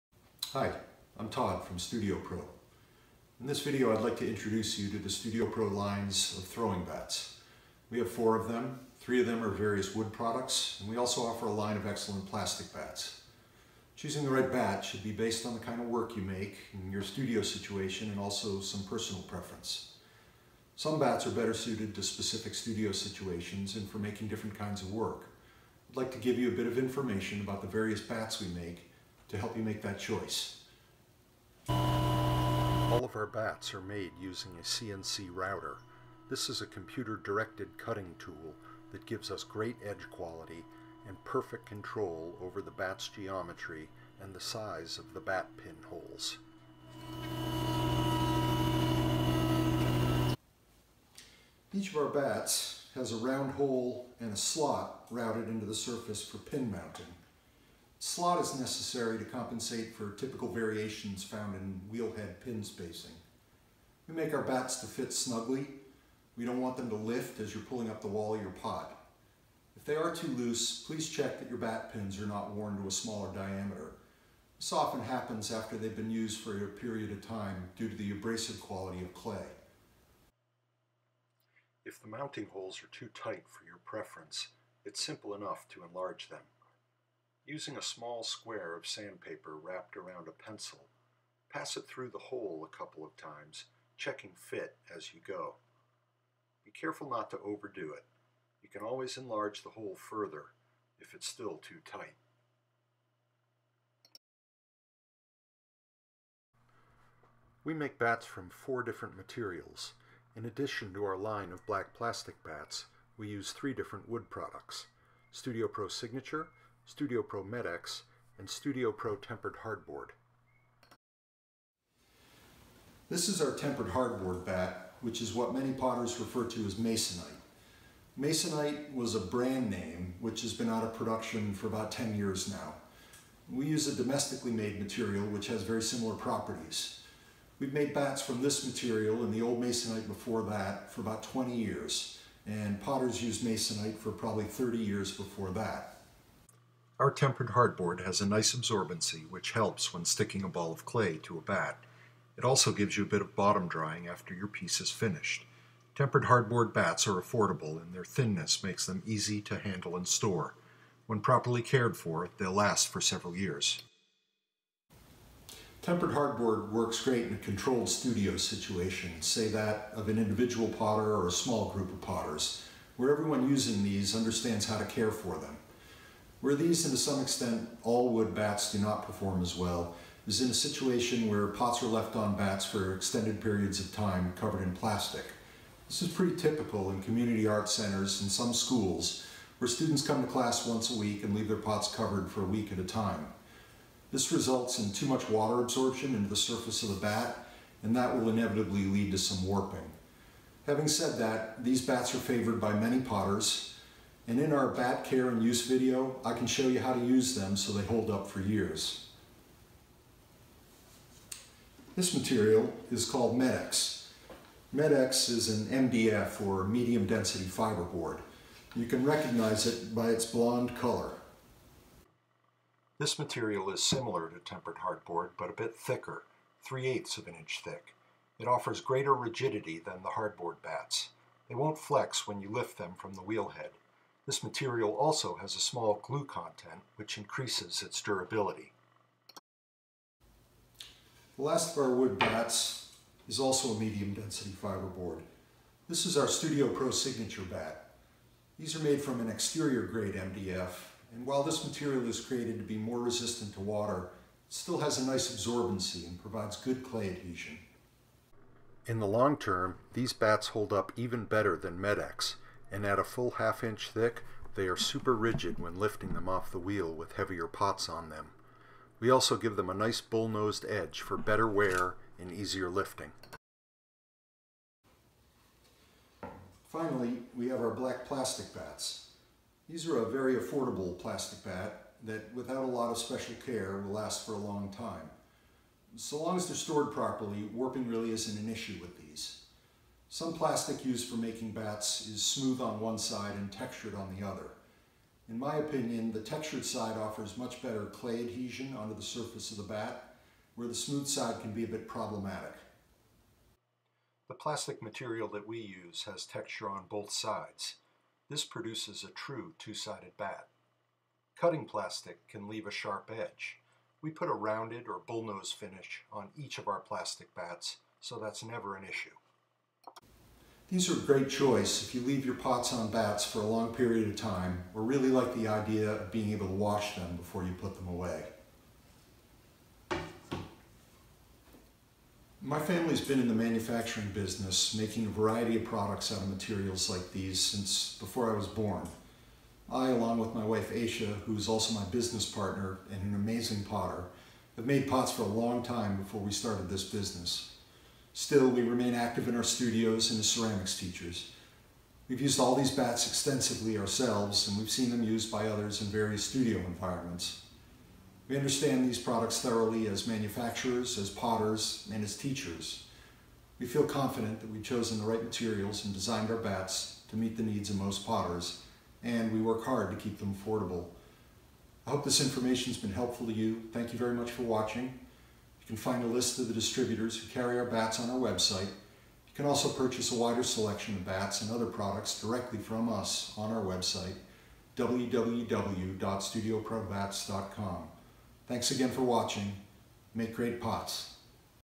An Introduction to StudioPro Throwing Bats
This video outlines the various materials, features and properties of the different StudioPro throwing bats. It provides an overview so that you can choose right bat for your work and studio.